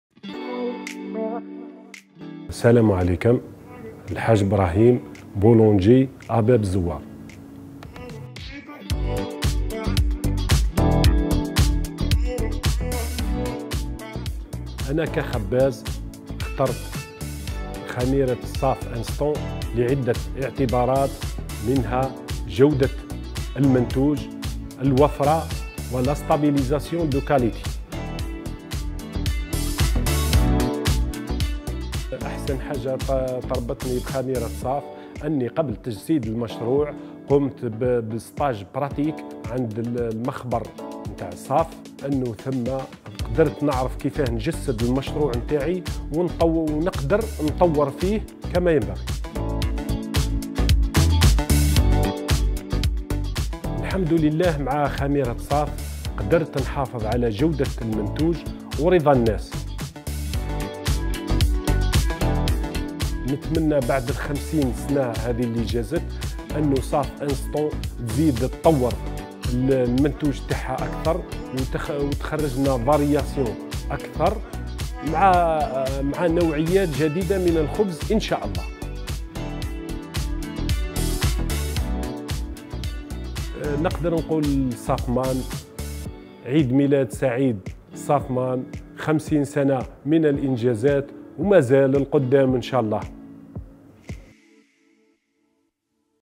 Interview Boulangerie à Bab Ezzouar
Les raisons qui ont poussé Hadj, Boulanger de métier, à choisir la levure sèche Saf Instant